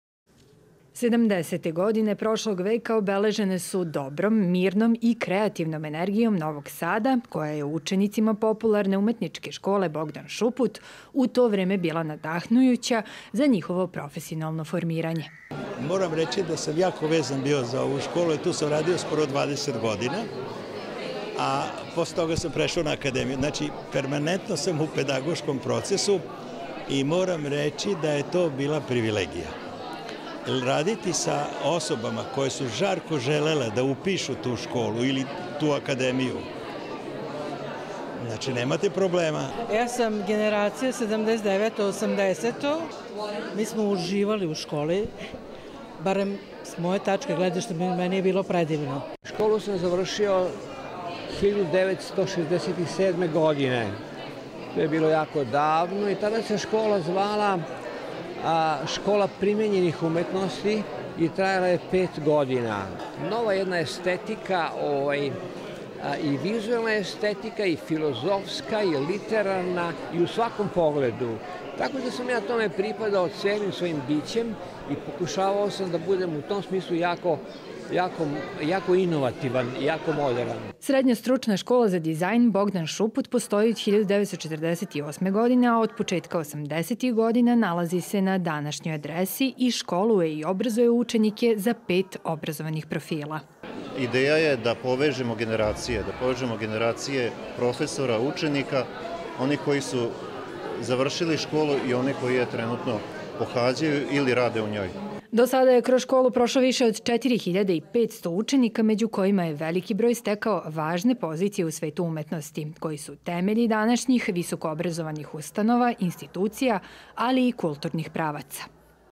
U susret obeležavanju 75 godina od osnivanja Srednje škole za dizajn"Bogdan Šuput"
U susret obeležavanju 75 godina od osnivanja Srednje škole za dizajn
"Bogdan Šuput" u Novom Sadu organizovana je izložba bivših učenika škole
iz generacije 1979/80.